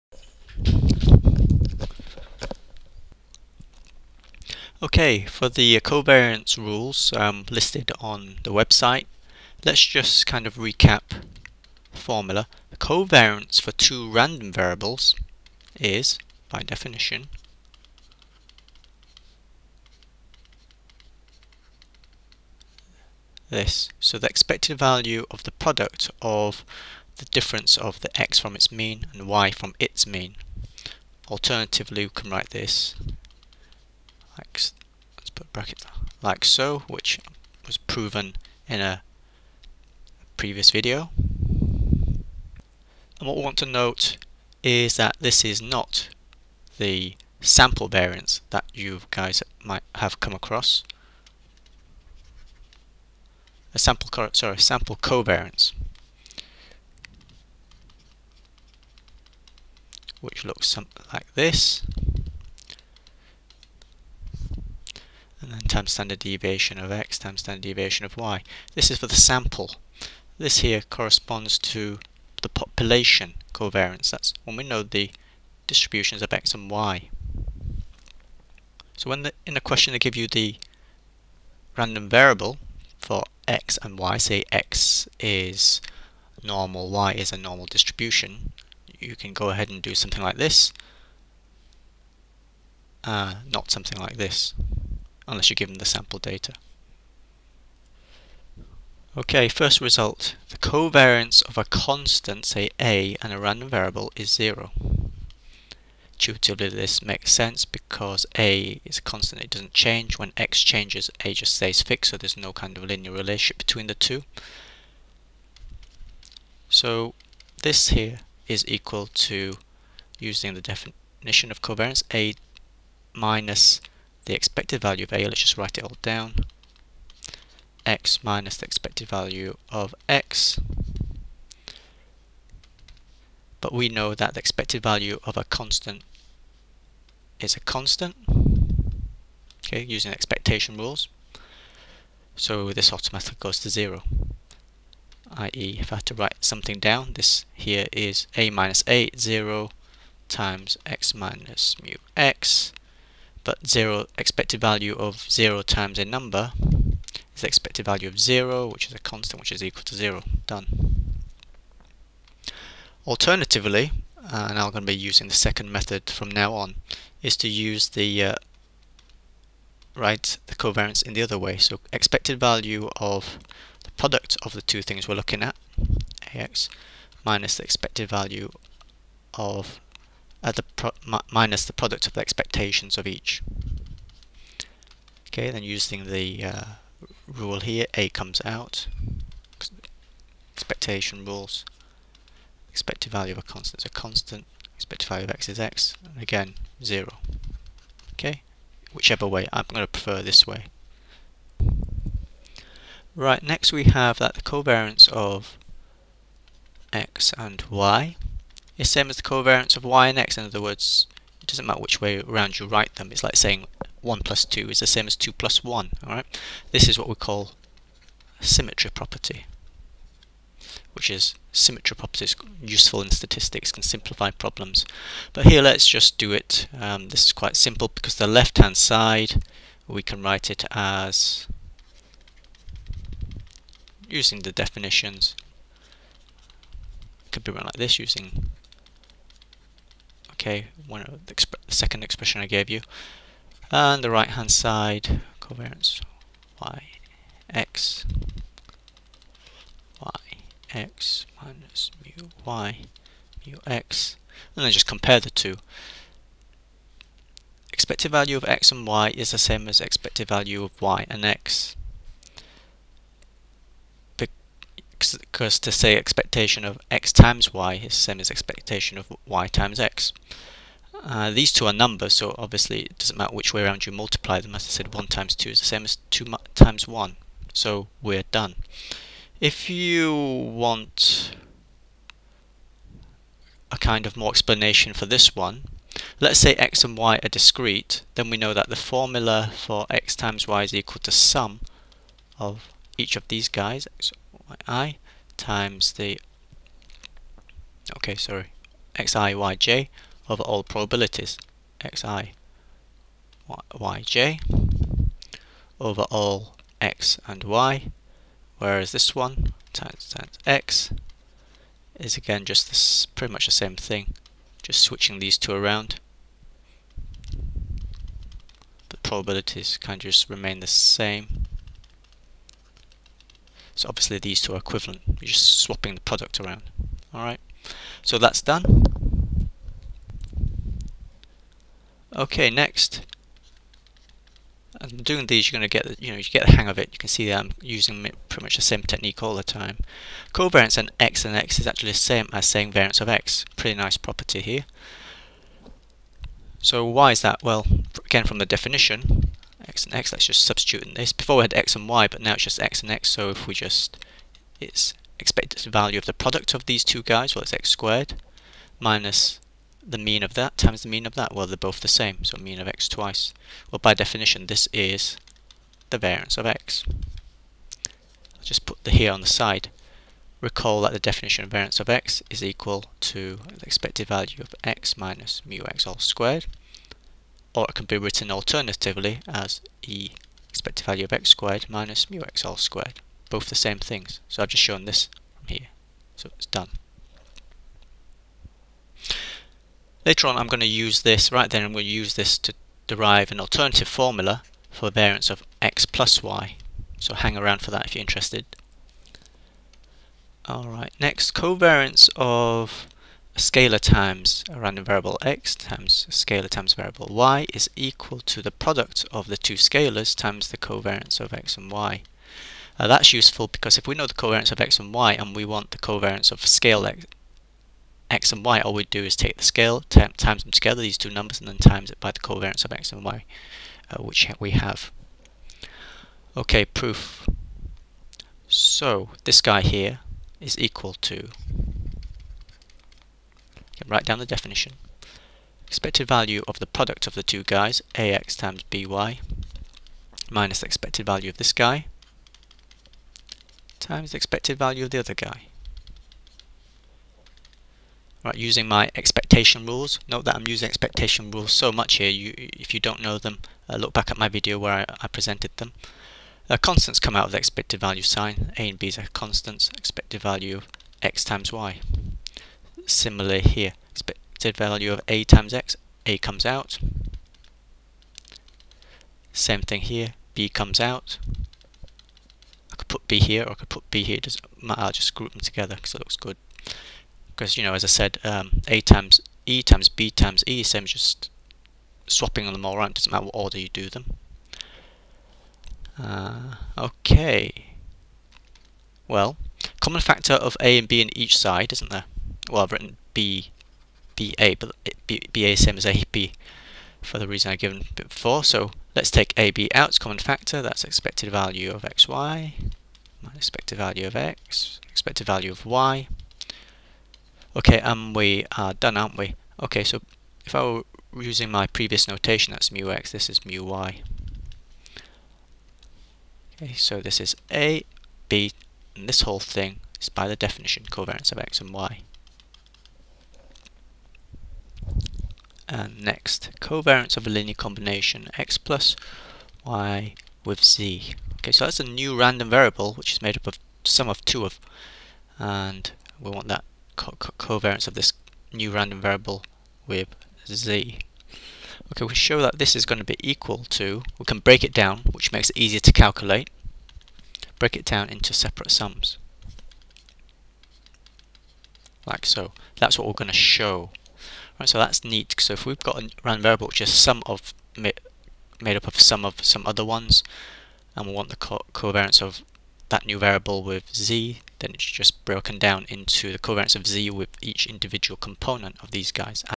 #85 Covariance properties with proofs part 1
Properties of covariance, and proof of var(x+y) using covariance rules.